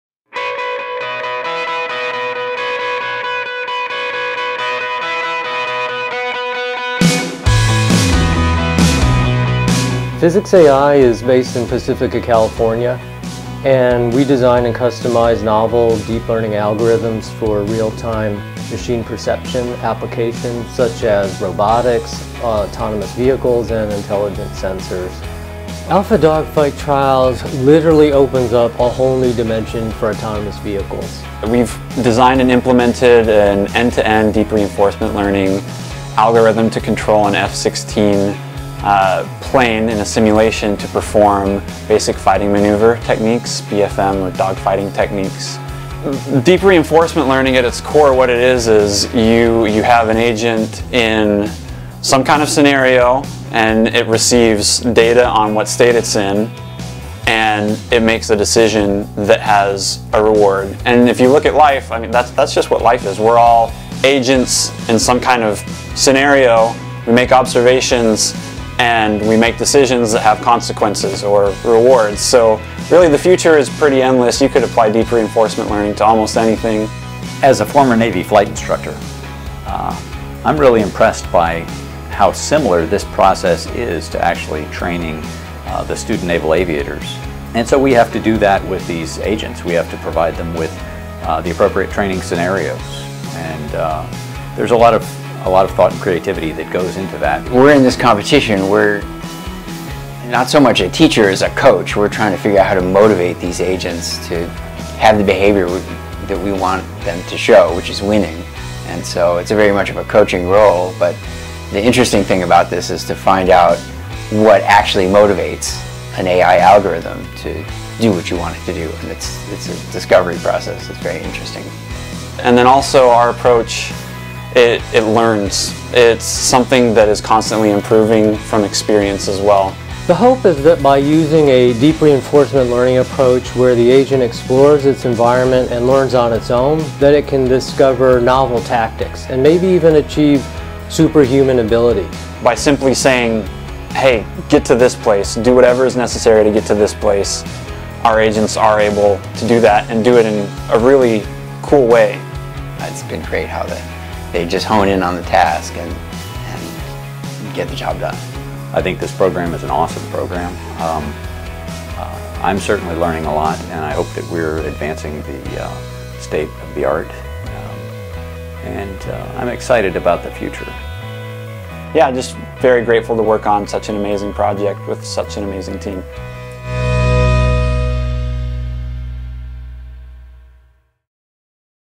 PhysicsAI AlphaDogfight Trials
Introduction to the PhysicsAI team competing in the DARPA AlphaDogfight Trials (ADT), a virtual competition designed to demonstrate advanced artificial intelligence (AI) algorithms that can perform simulated within-visual-range air combat maneuvering, colloquially known as a dogfight.

The PhysicsAI autonomous agent is trained from scratch with no prior knowledge, and over thousands of flights learns on its own how to fly and perform basic fighter maneuvers (BFM) using end-to-end Deep Reinforcement Learning.